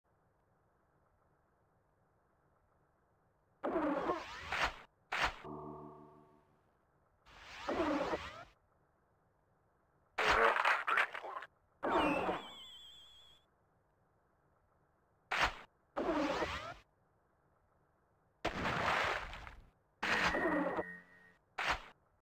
Tele-splat
Poor timing going through a teleporter on Cov's part.